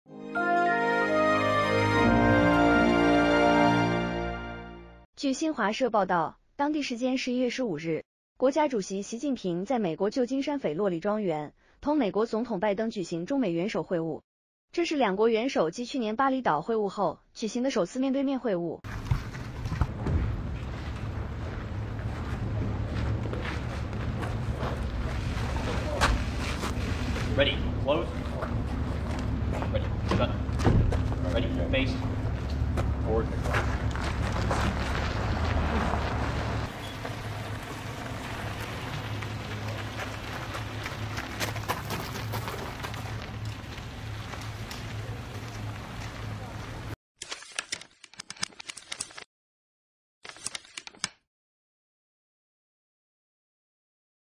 中国国家主席习近平抵达斐洛里庄园同美国总统拜登会晤
据新华社报道，当地时间11月15日，国家主席习近平在美国旧金山斐洛里庄园同美国总统拜登举行中美元首会晤。这是两国元首继去年巴厘岛会晤后举行的首次面对面会晤。